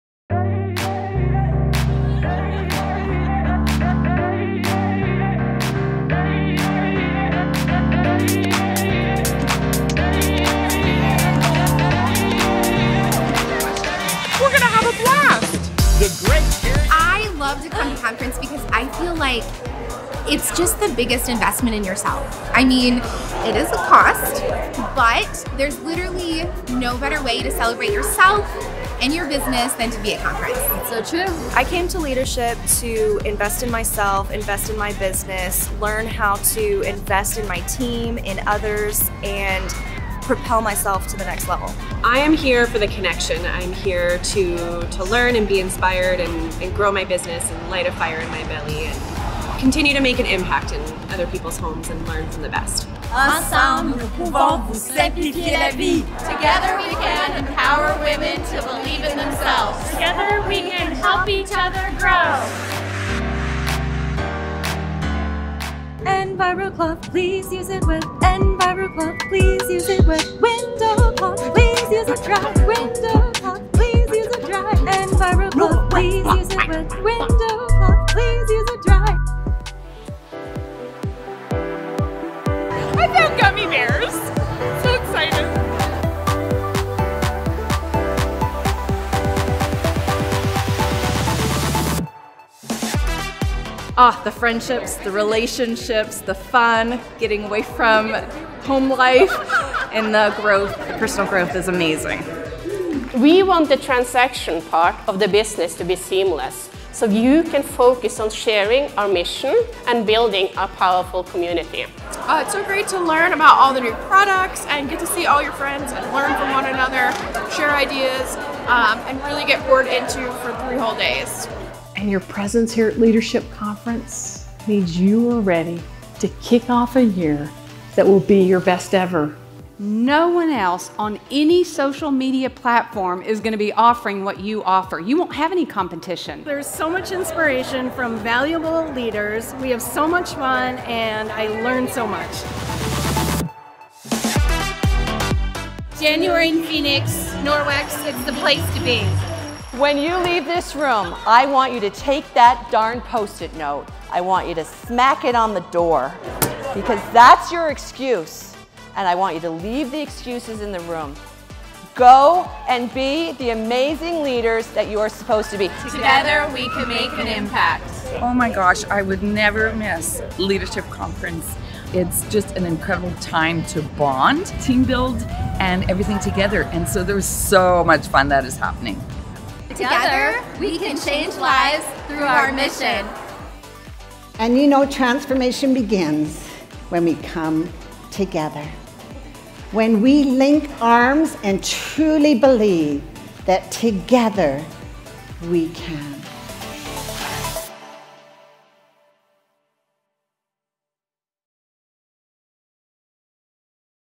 Norwex Leadership Conference 2023 Memories
We're feeling incredibly inspired after our 2023 Leadership Conference!

This past weekend we were reminded that everything is better when done together! Most importantly, together we can encourage and inspire others to create Safe Haven's in their homes.